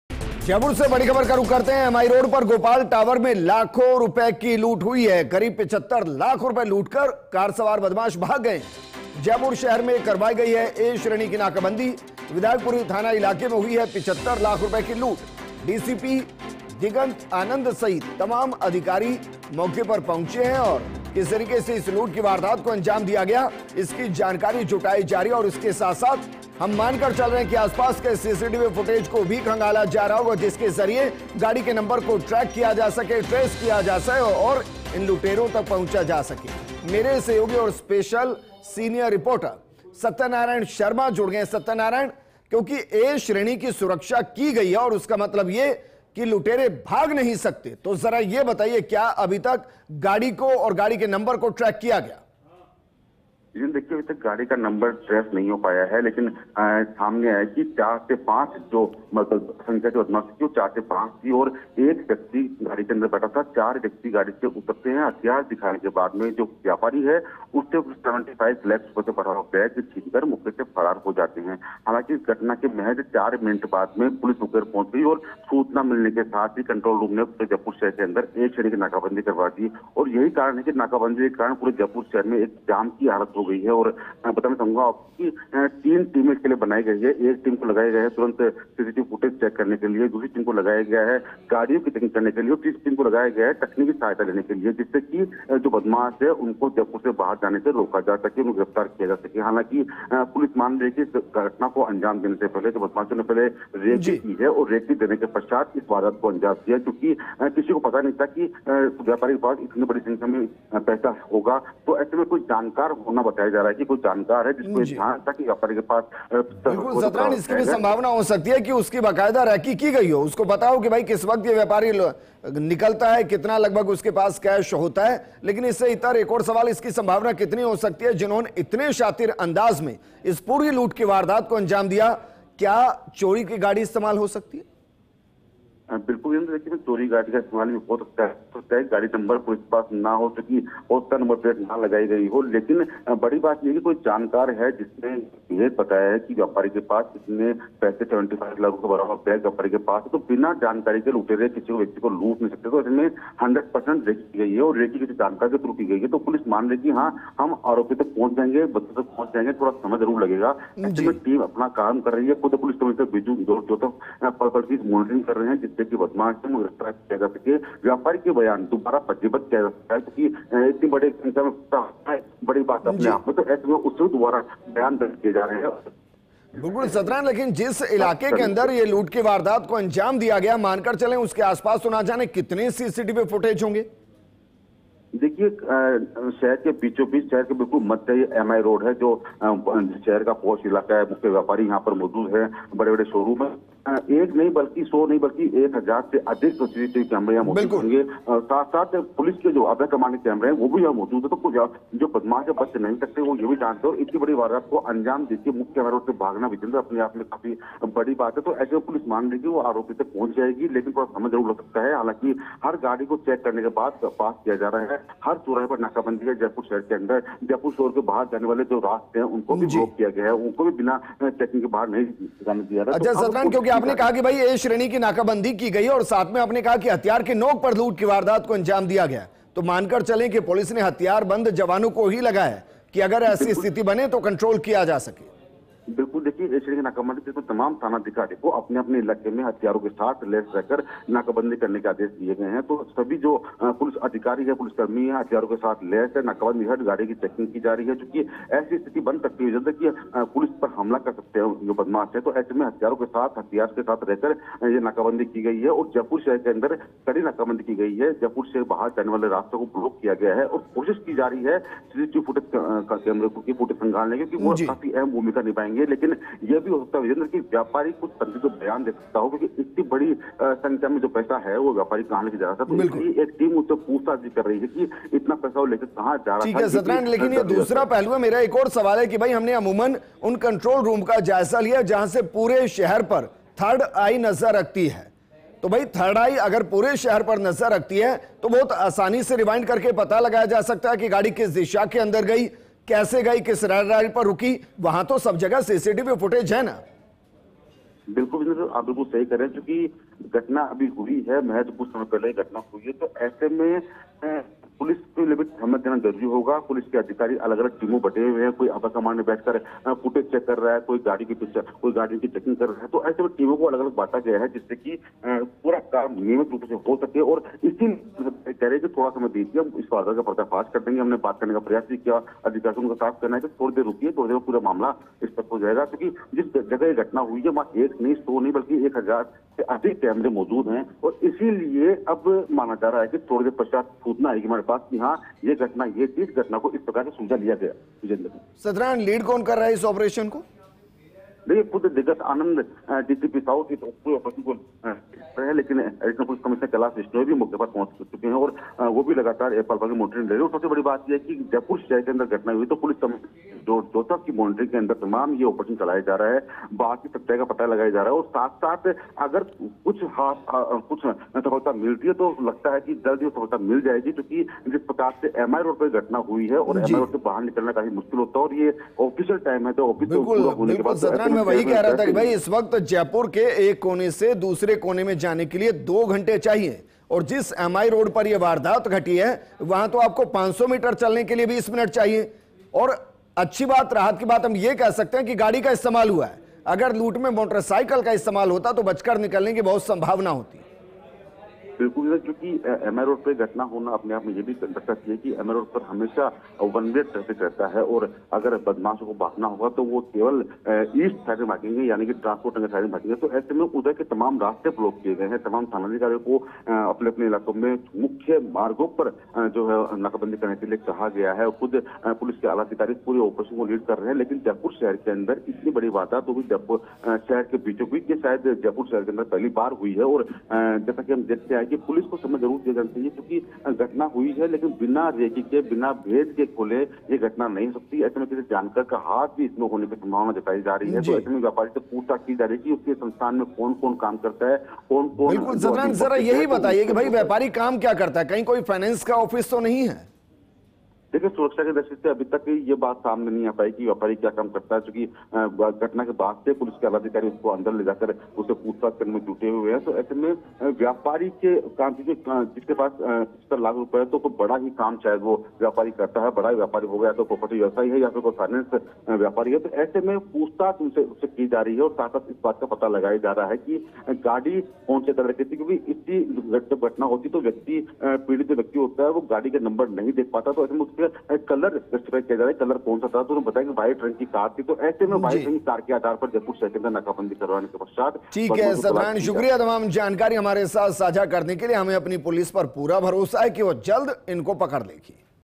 गोपाल टावर में लाखों की लूट, DCP सहित तमाम अधिकारी मौके पर मौजूद | Jaipur News | Robbery Case
About this Video:
जयपुर से बड़ी खबर, MI रोड पर गोपाल टावर में लाखों रुपए की लूट, करीब 75 लाख रुपए लूटकर भागे कार सवार बदमाश, जयपुर शहर में करवाई गई A श्रेणी की नाकाबंदी, विधायकपुरी थाना इलाके में हुई है 75 लाख की लूट, DCP दिगंत आनंद सहित तमाम अधिकारी मौके पर | 24 June 2024